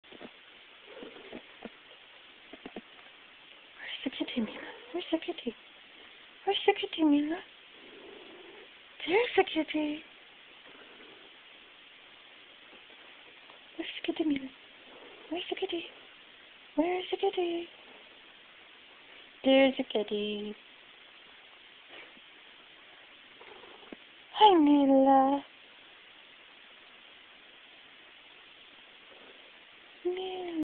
tuxedo cat Mila and the hijab
I have a large hijab on and I cover Mila my tuxedo cat with it and get her to uncover herself